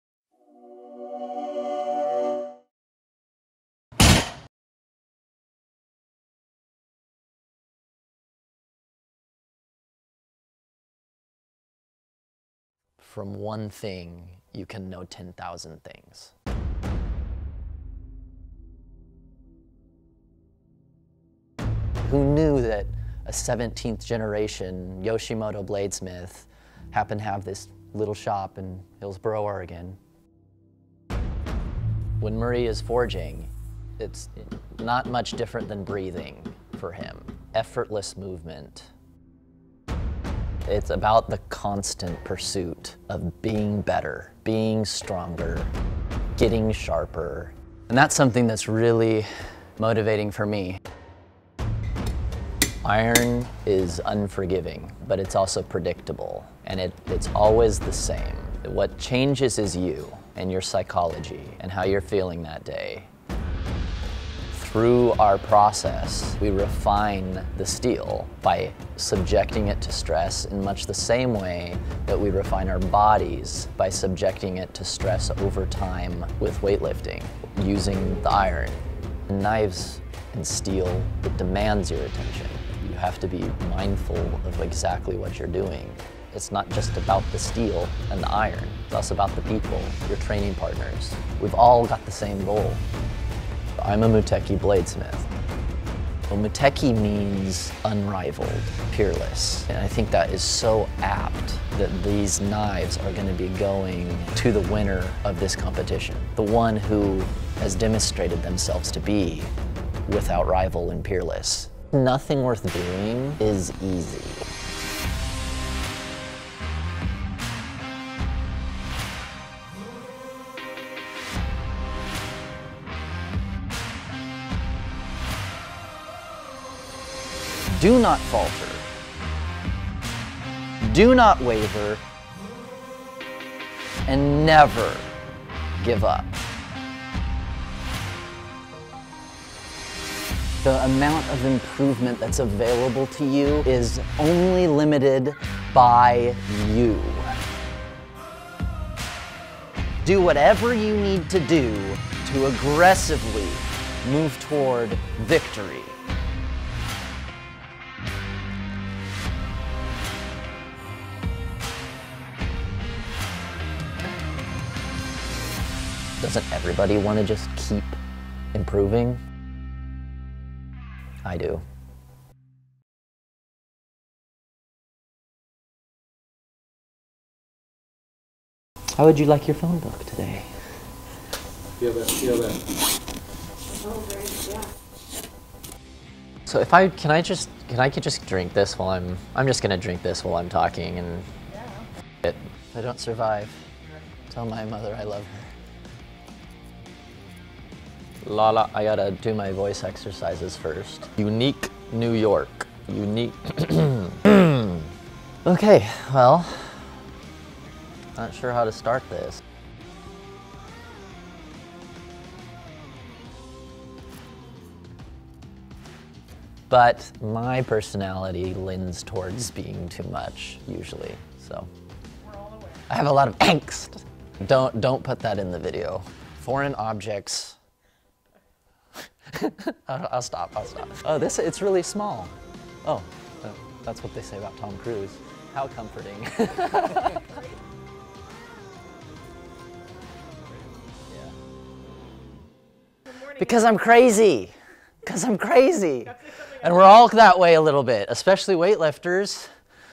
Muteki: Forging Body and Blade
Muteki Bladesmith Taylor Shields draws parallels between bladesmithing and weightlifting while training for a US Strengthening Championship.

The same competition that he made the winning prize knives for!

Watch as he creates his blades, tells his story and trains for the torment set to kick off TOMORROW!

Good luck to all the competitors, and congratulations to those who win a Muteki knife!